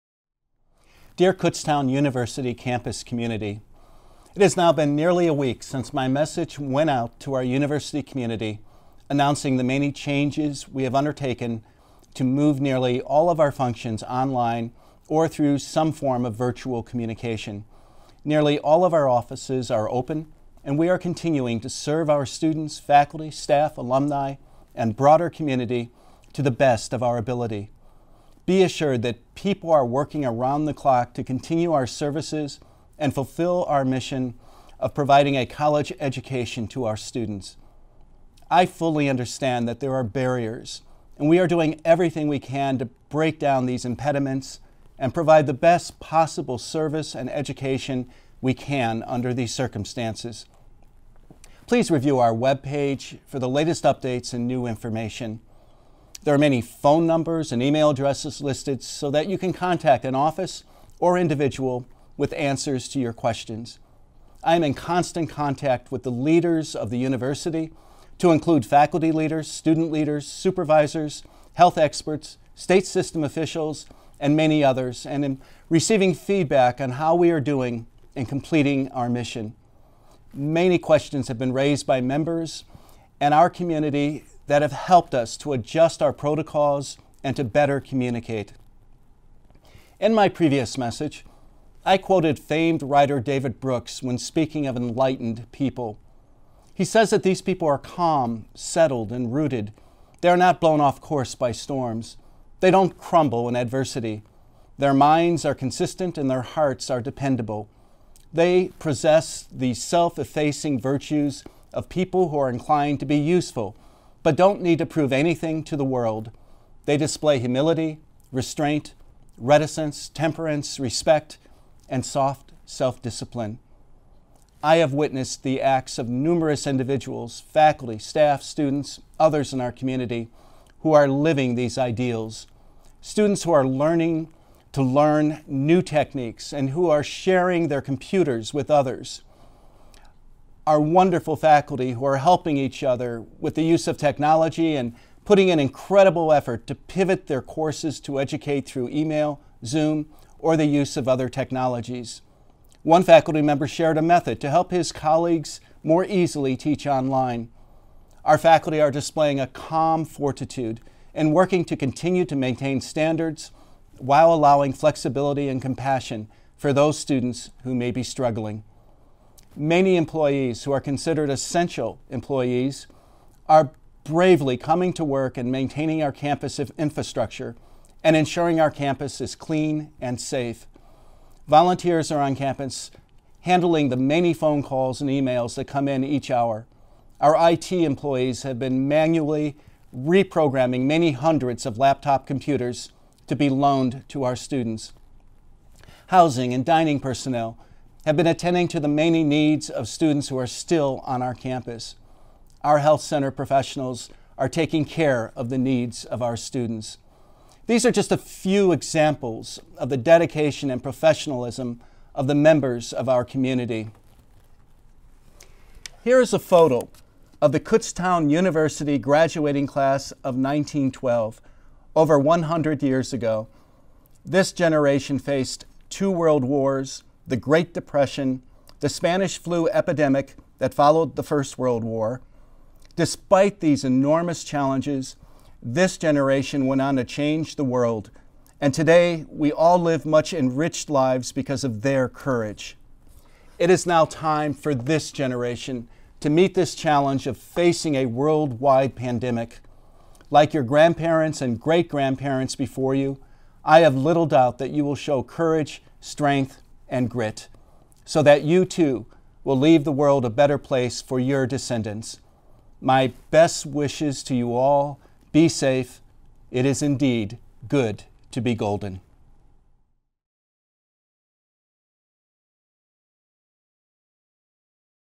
Update from President Hawkinson
Dr. Kenneth S. Hawkinson, President of Kutztown University, addresses the university community as KU prepares to transition to online classes for the remainder of the spring semester.